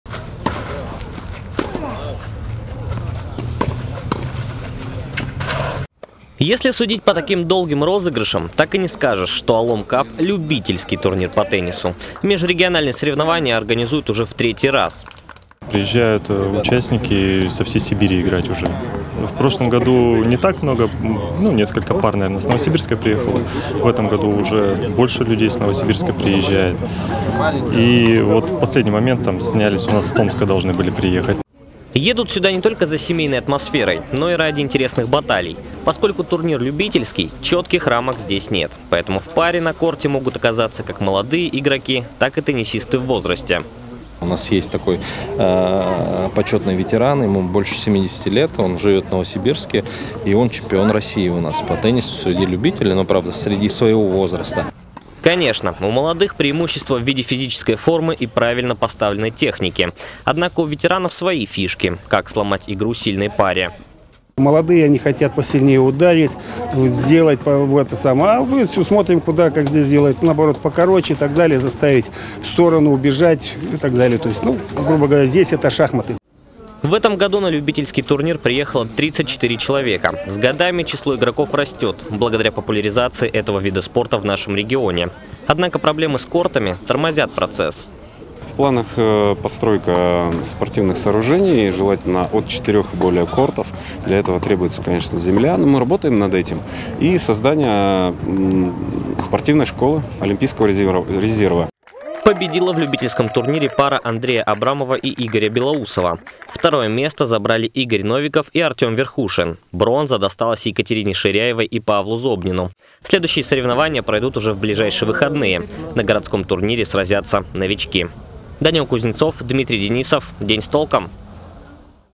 В День города прошел любительский турнир по теннису
Межрегиональные соревнования по теннису  в Барнауле  в День города собрали свыше 30 игроков. Организаторы отмечают, что в сравнении с прошлым годом- участников стало значительно больше -приехали спортсмены из других регионов.   Поскольку соревнования любительские, на кортах встречались спортсмены разных возрастных категорий. О том, что оказалось важнее опыт или молодость? – смотрите в сюжете.